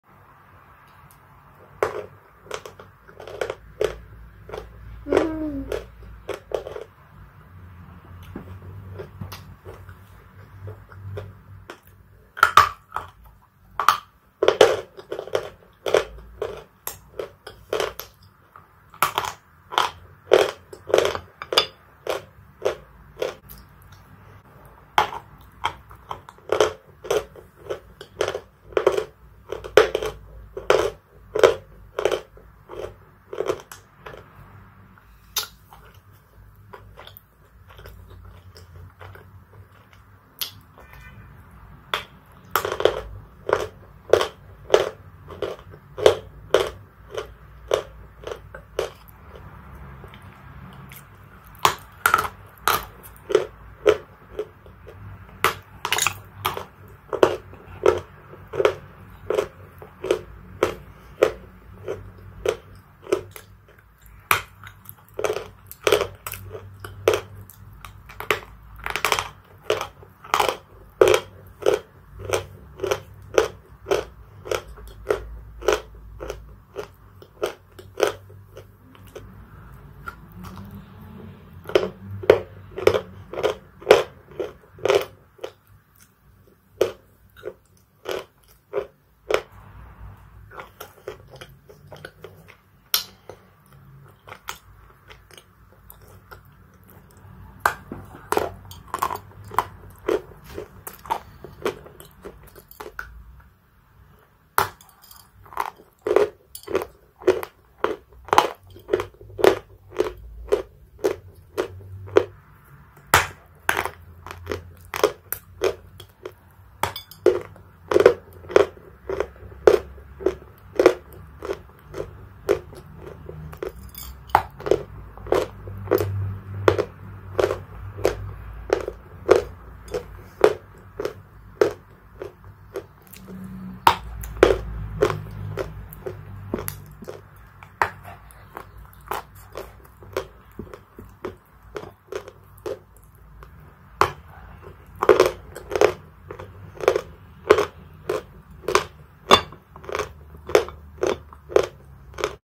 VlogAyza_Tagay Eating🍽🍺wet blue Uzbek💦🥤chalk📏🧃 in blue Uzbek cream🧁🥂Turkistan clay🤎🍸
❌🚫 don’t swallow 🚫❌
Eating chalk,clay or asmr is bad for health 🧠🫁🫀
Just watch the video and be relax 💆🏻‍♂️🧘🏼‍♂️☺️
Check my Instagram too if like more videos of crunch